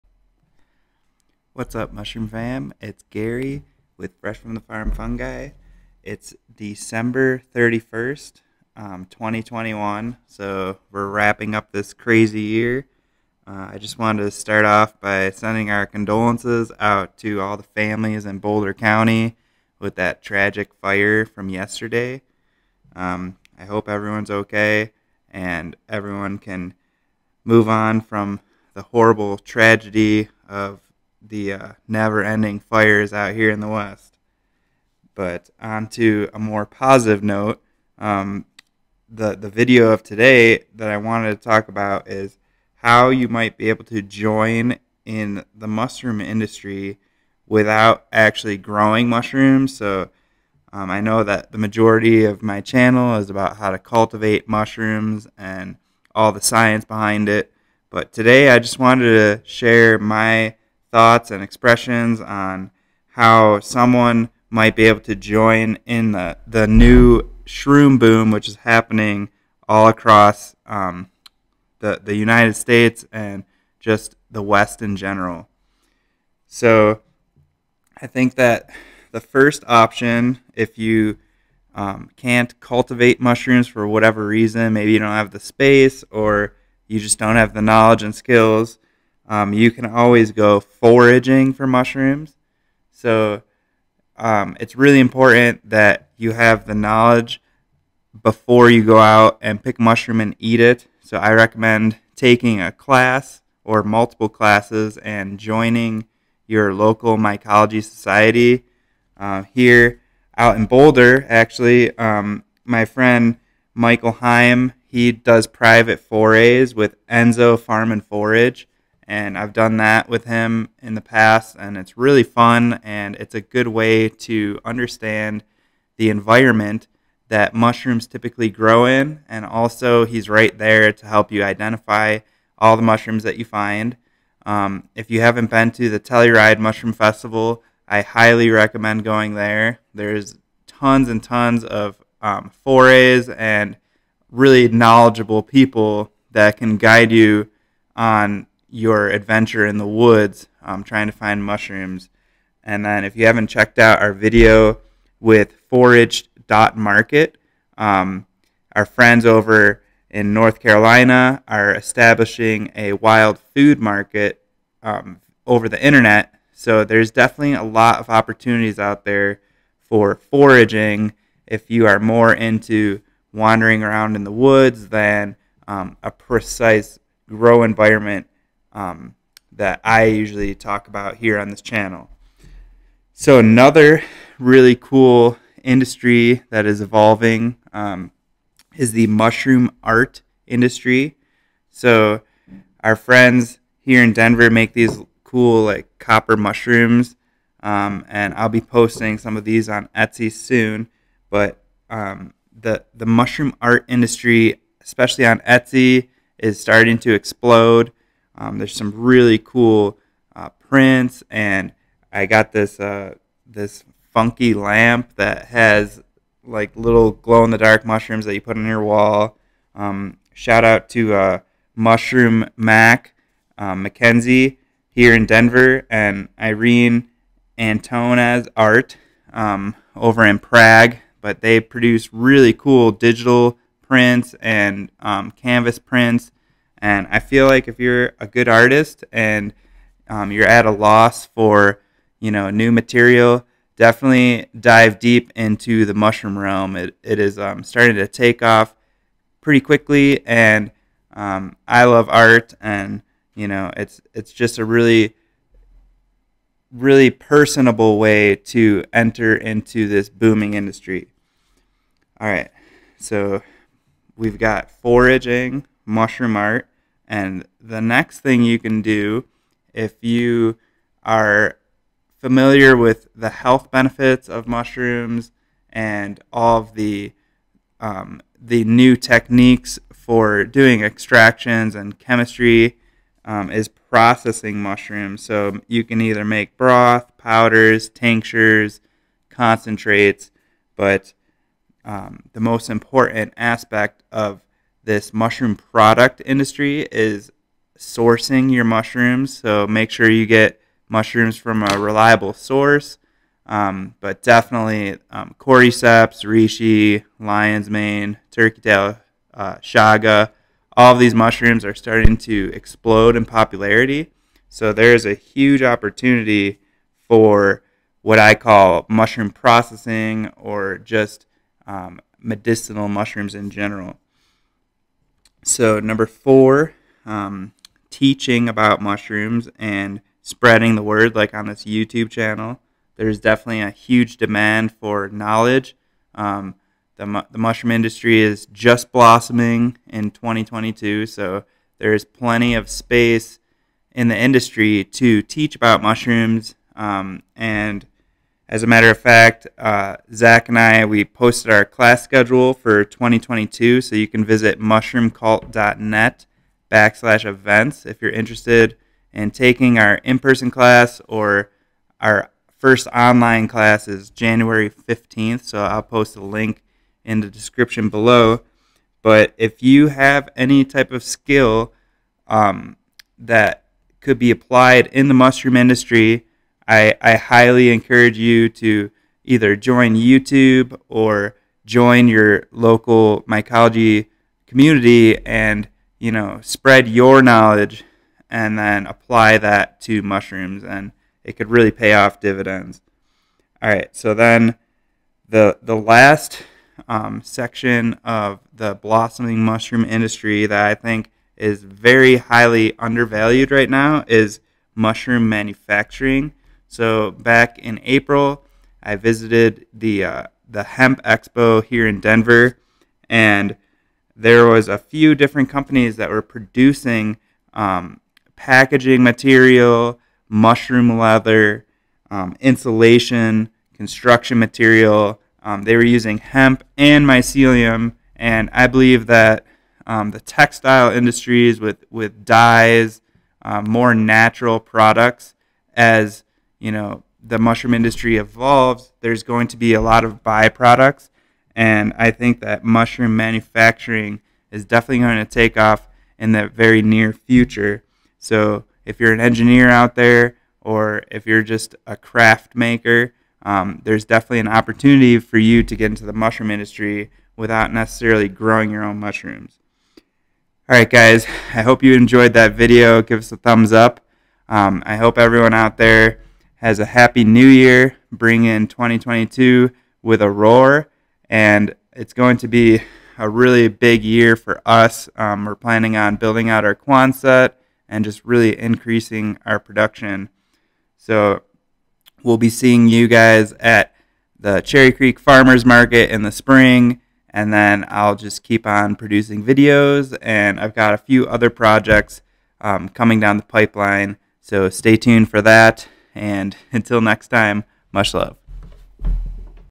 Mushroom Industry Opportunities in 2022! Shroom Boom
Happy New Year Mushroom Fam! Today I wanted to talk about all the opportunity in the blossoming mushroom industry!

Gary’s Favorite Mushroom Gear

MUSHLOVE